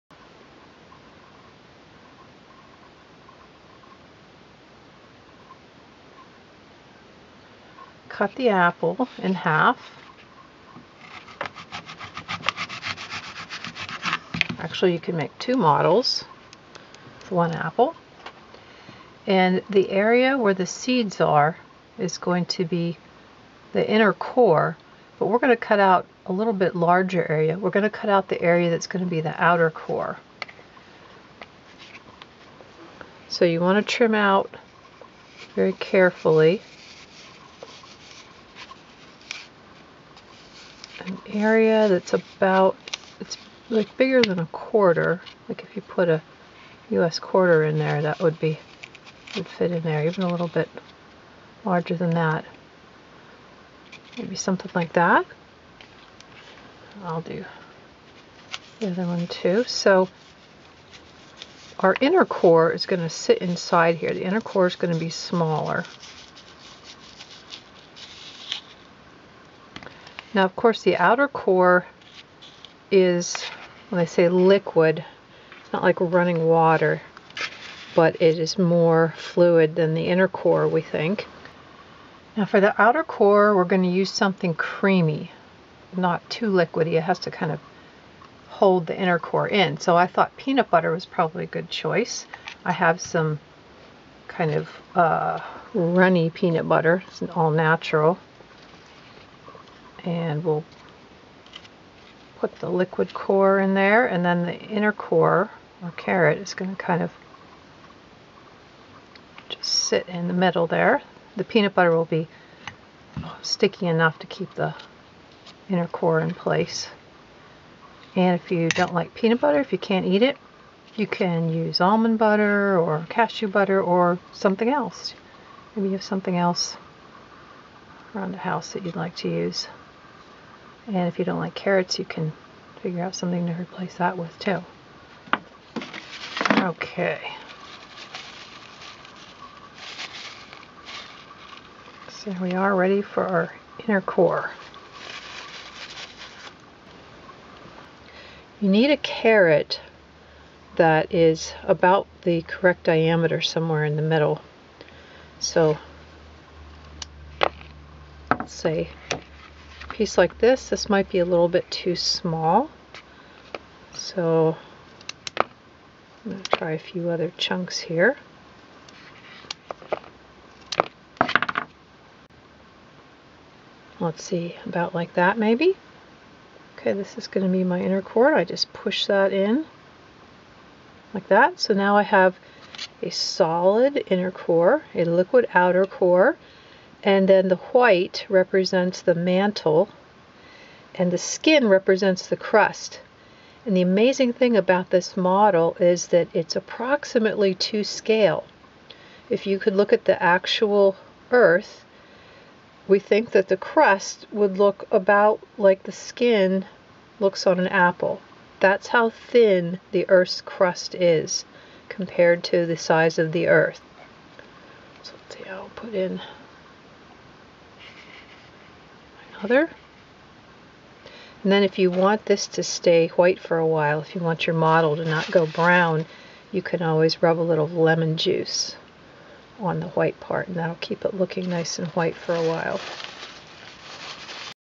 Apple model of earth
Turn an apple into a model of the inner earth